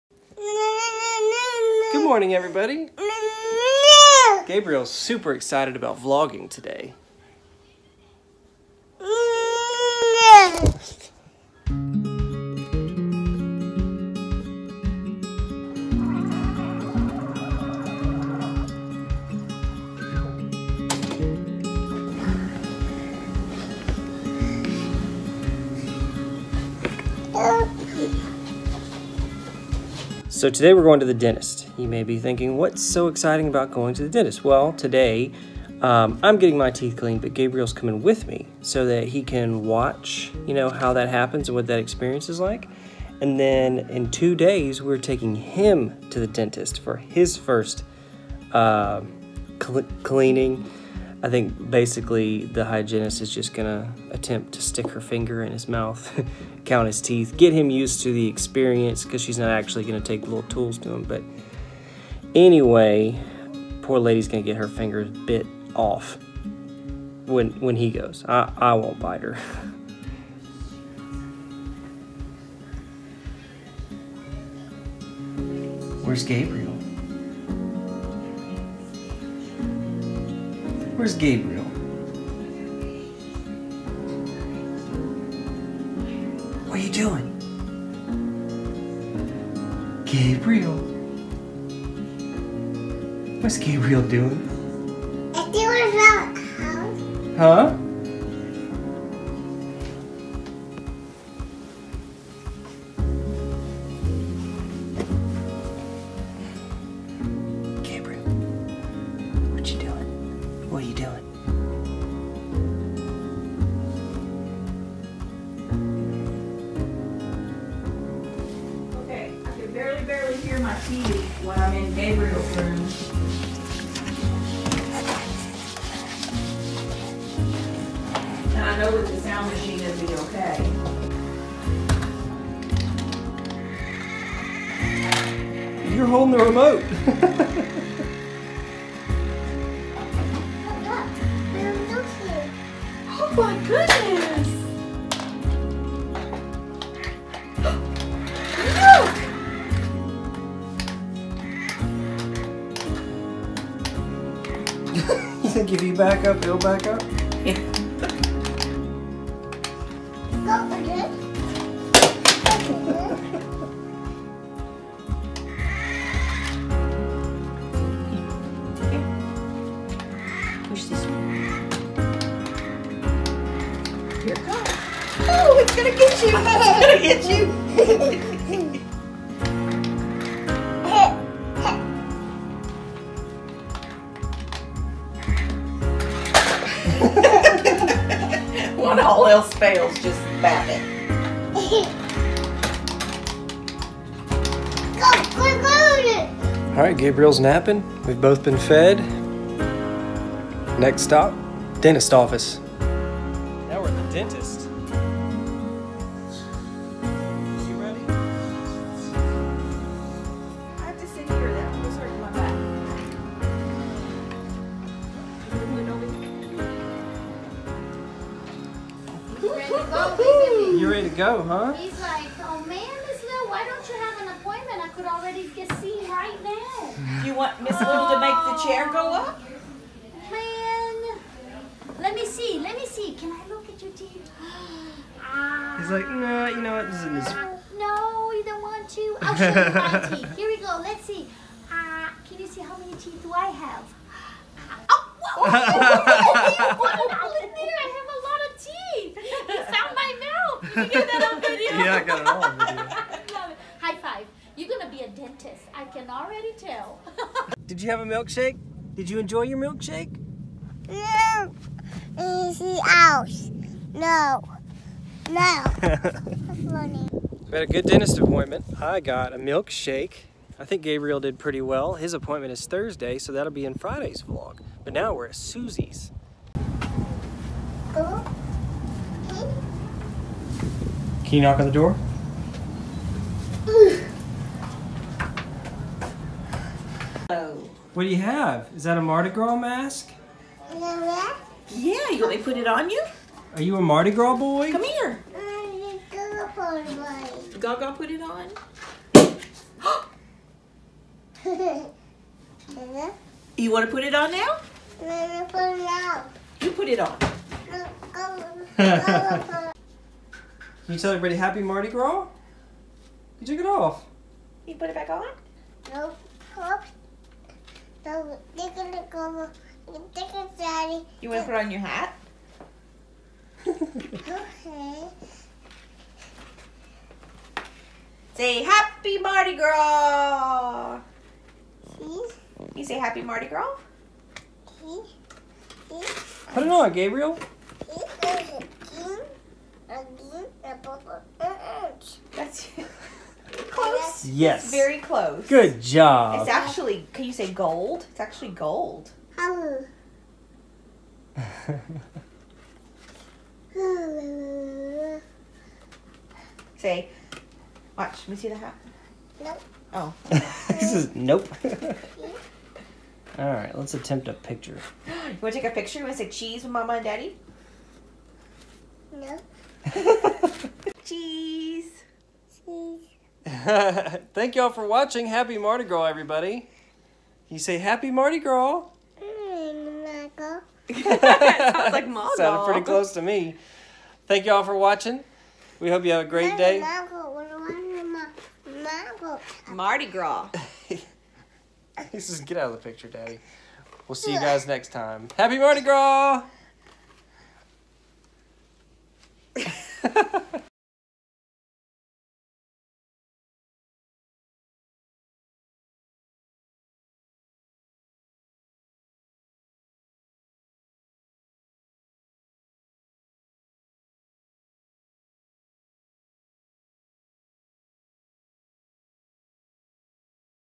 Spending Mardi Gras At The Dentist's Office
Vlog # 248

It's Mardi Gras in the Upshaw house! Unfortunately that doesn't mean much this year. Today Jacob got his teeth cleaned and Gabriel came along so he could see what it was like before his first dentist appointment on Thursday! He seems ready!